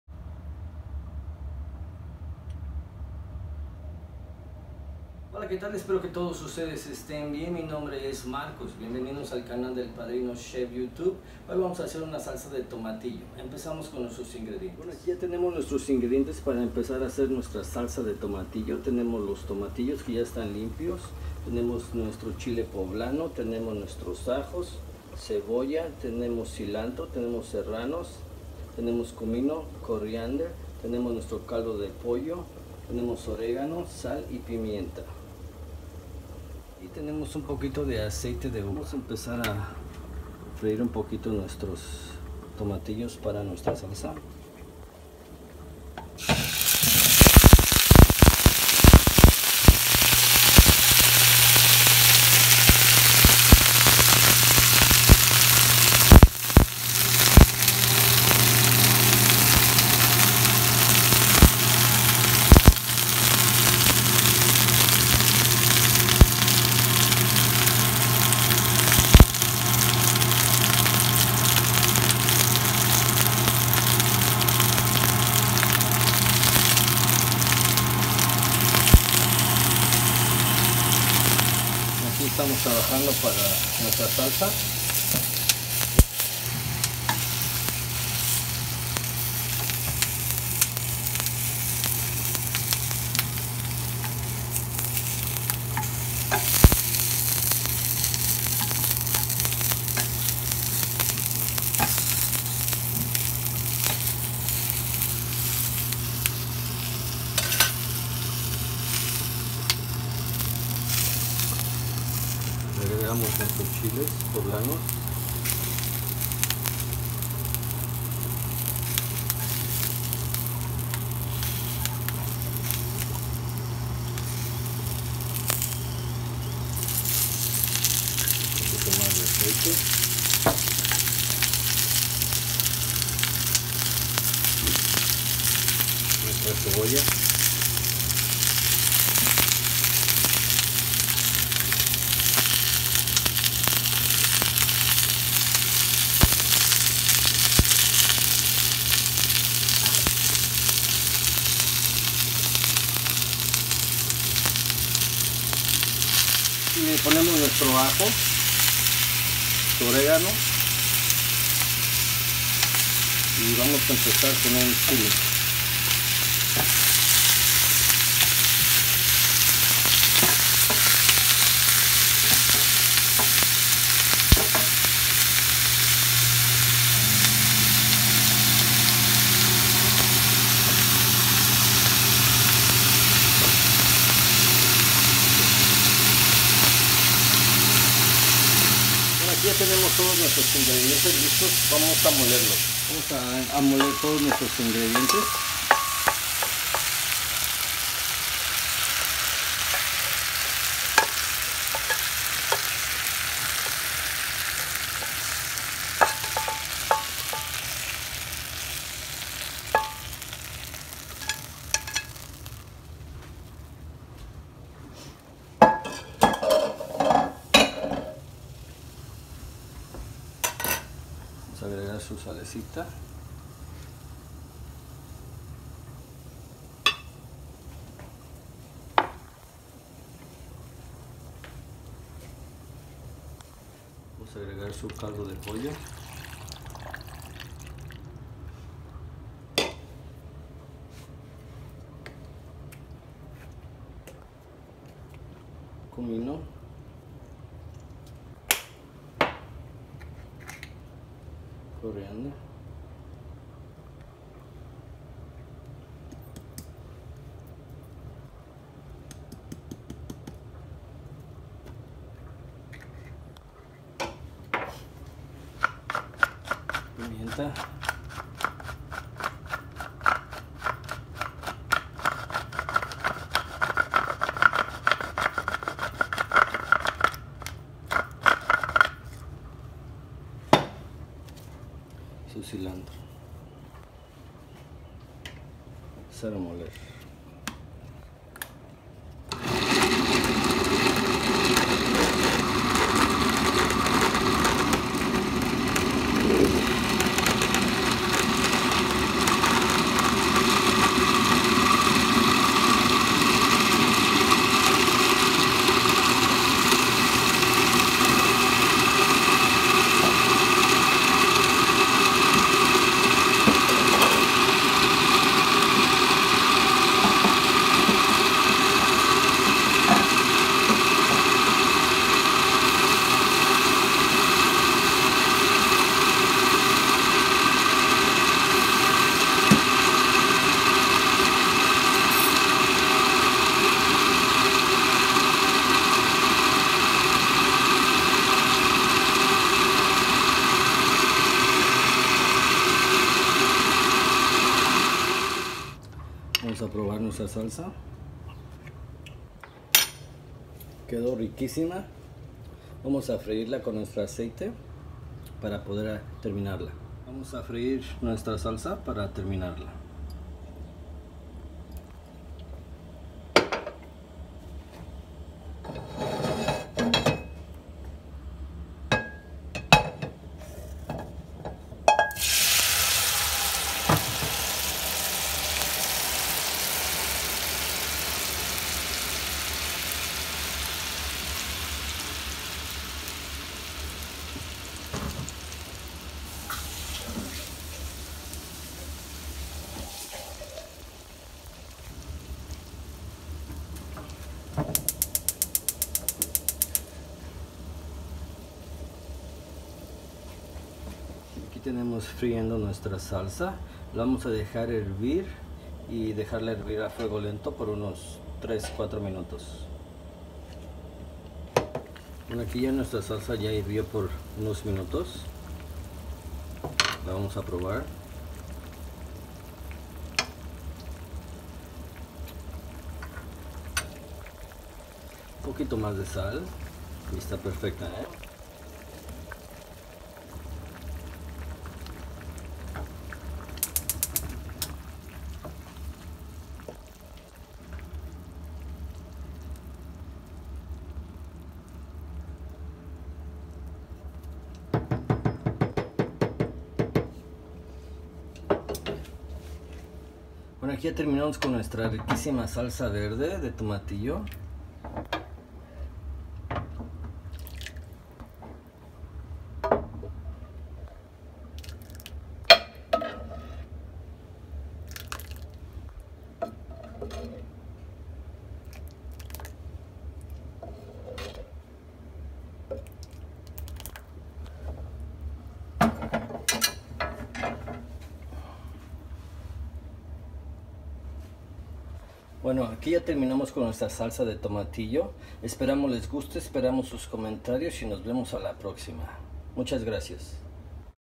Homemade Salsa de Tomatillo - El Padrino Chef
El Padrino Chef
Amazing news! We are so excited to share our recipes.
From our kitchen to yours.

Yields 28 oz
Homemade Salsa de Tomatillo
   • 1 1/2 lb tomatillos dice in quarters
   • 1 ea green bell pepper clean cut
   • 3 ea garlic cloves
   • 1/4 yellow onion small
   • 26 springs fresh cilantro
   • 2 ea serranos chili fresh
   • 8 oz chicken stock
   • 1/4 tsp cumin
   • 1/4 tsp coriander
   • 1 tsp oregano dried
   • 1/2 tsp kosher salt
   • 1/4 tsp black pepper fresh

We hope you enjoy this video and share it to keep our Mexican cooking tradition.
Gracias,
El Padrino Chef